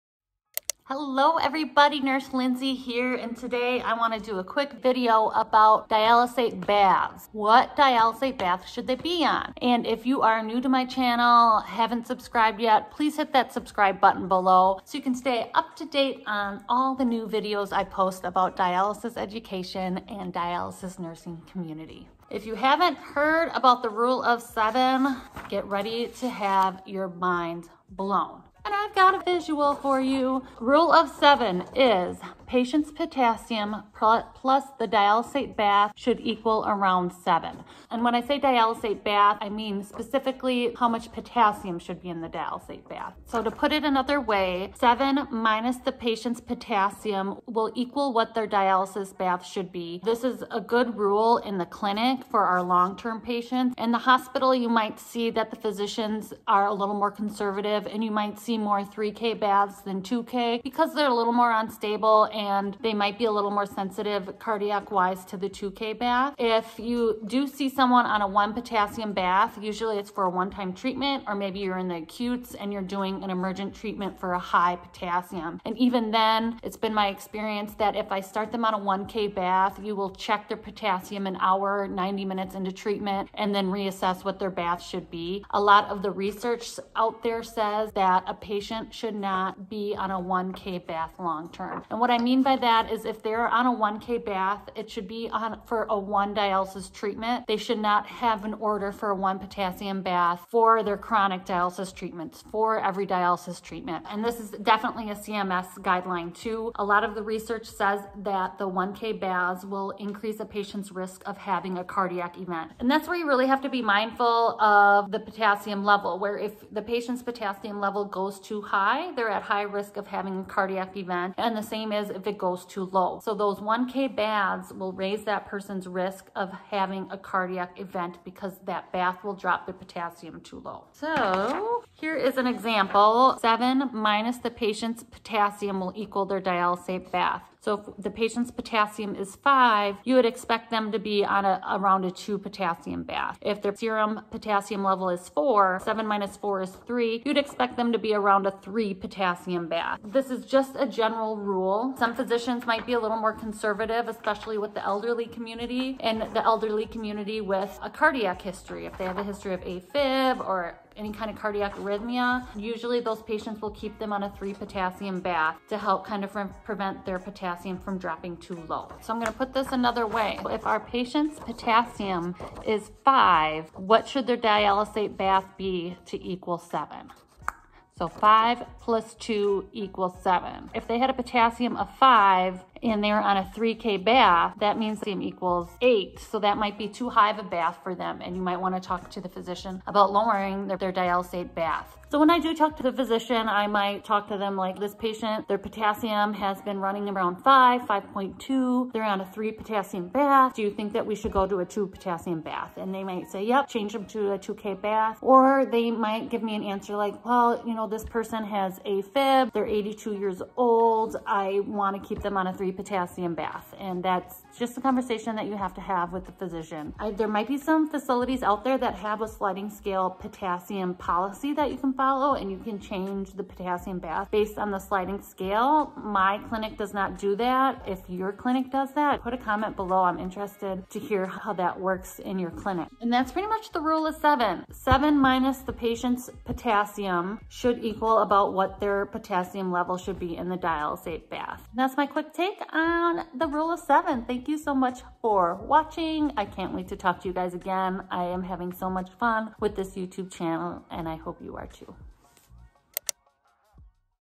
Dialysis Nursing: Rule of 7 and Potassium in Dialysate
Understanding the amount of Potassium in the Dialysate Bath is clear and simple when you understand the Rule of 7.

Giraffe Sweatshirt

Patient's Serum Potassium (K) + Potassium (K) in Dialysate = ~ 7
Or
7 - Patient's Serum Potassium (K) = ~ Potassium (K) in patient's dialysate bath

This Rule of 7 will help you talk to you physician about the patient's most appropriate dialysate bath.

This is a general rule, and there are always exceptions to the rule.

00:00 Rule of 7 Intro
00:50 Rule of 7 for the hospitalized patient
01:10 1 K bath and dialysis
02:10 Rule of 7 again
03:00 Talking the the provider about your dialysis patient's dialysate bath